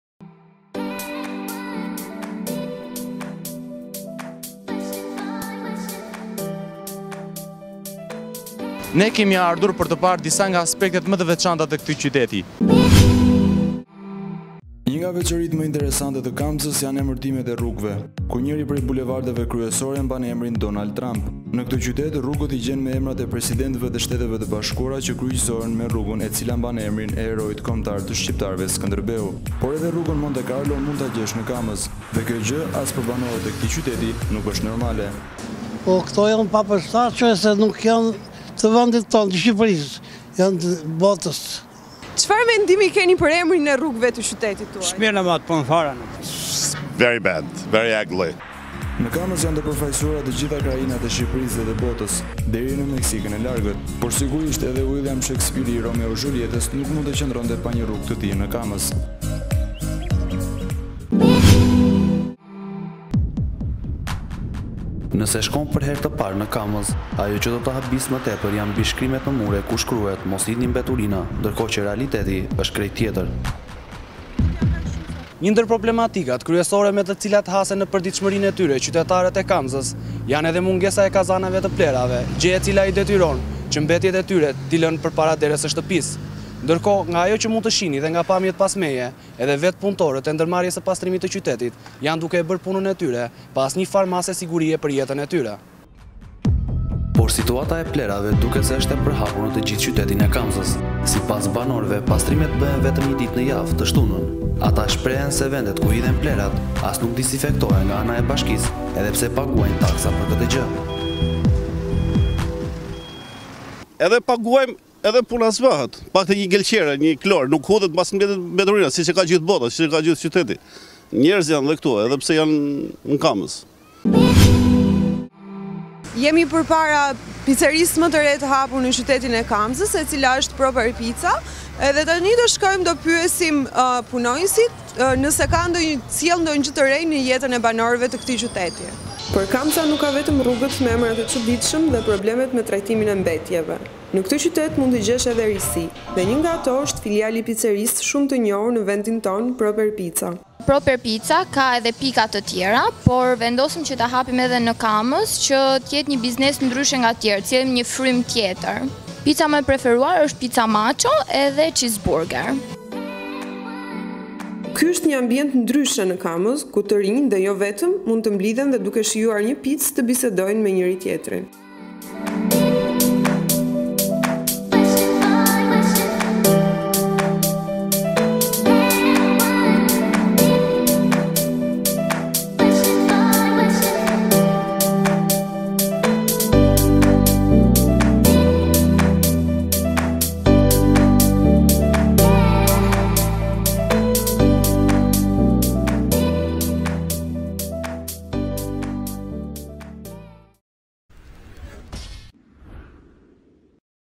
3në3 - Kamza
Në rreth 3 minuta ju tregojmë Kamzën, në një video ndryshe nga ato që mund të keni parë deri më sot! Ju tregojmë vendin që lidh Donald Trump me Shekspirin! Xhorxh Bush me Skënderbeun! Por edhe dy elemente të tjera.

Rubrika 3 në 3 është produksioni më i fundit i Qendrës për Gazetari Cilësore. E ndërtuar nga 3 elemente kyçe “Fenomeni”, “Problemi” dhe elementi i fundit “Tek vendi”, një vendndodhje speciale.